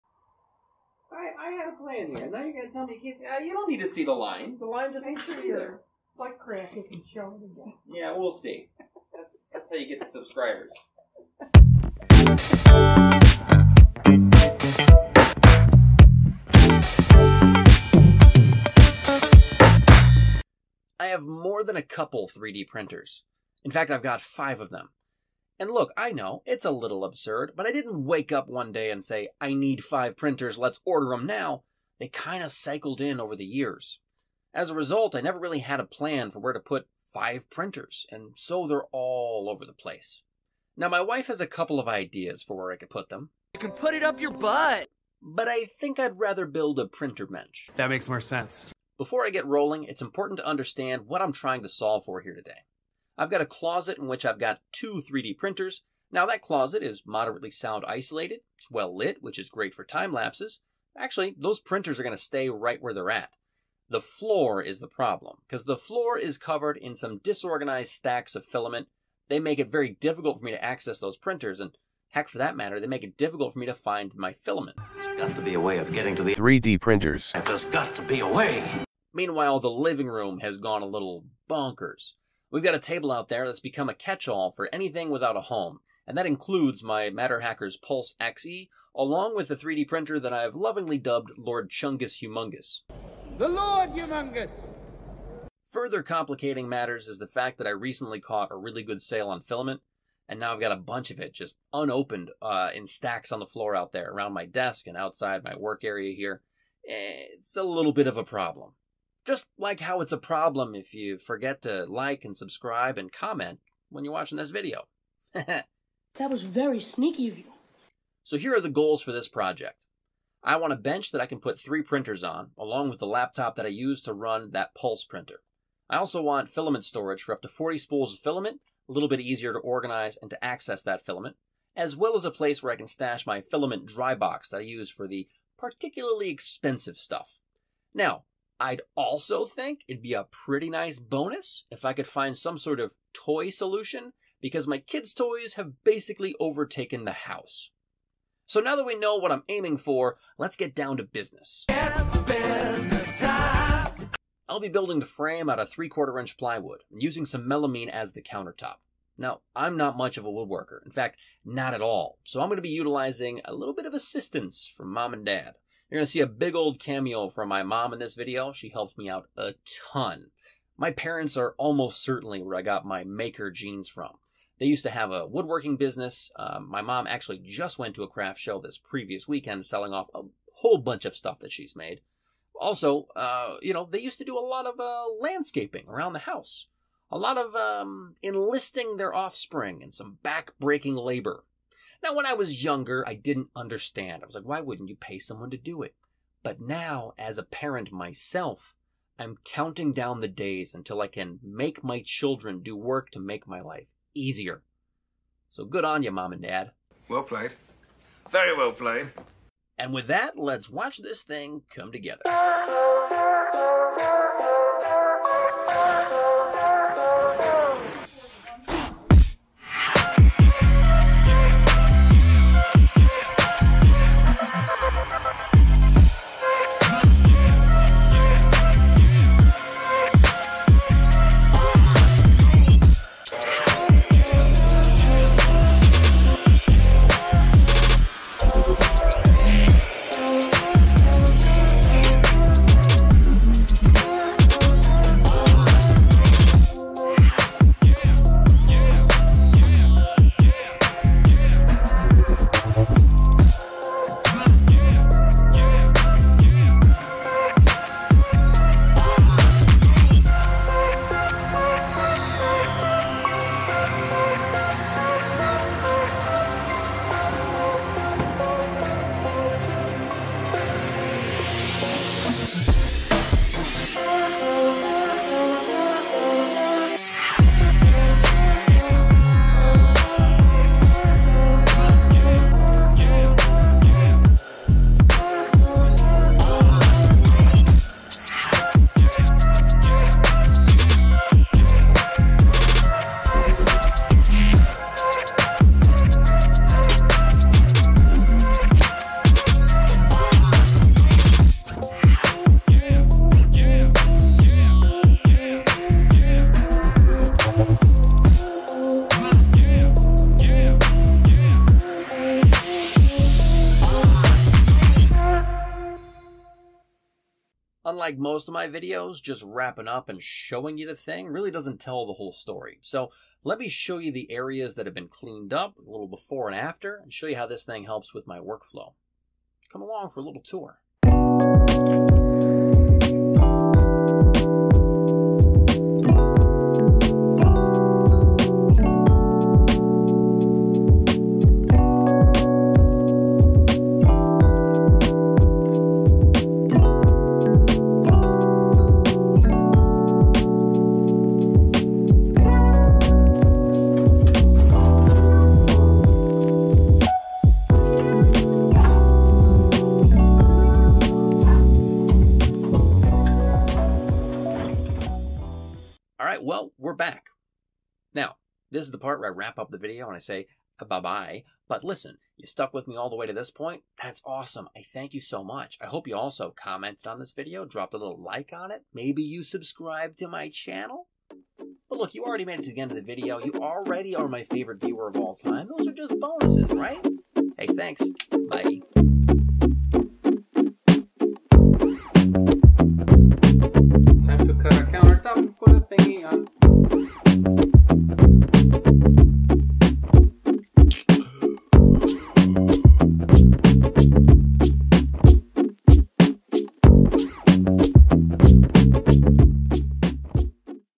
Building the Ultimate 3d Printer Bench
3d printers had spread as weeds in a field. Spires of filament rose from the earth to form pillars of bioplastic. Entire swaths of table and floor space were lost beneath the waves of flush cutters and calipers, buildplates and glue sticks, chaos and disarray.

So I built a 3d printer bench out of 3/4" plywood and some melamine shelving from Home Depot.

3d Printers:

Robo3D R1+
Matterhackers Pulse with Olsson Ruby nozzle
Monoprice Mini Delta
Monoprice Maker Select Plus
Lord Chungus Humongous (heavily modified Geeetech A20M)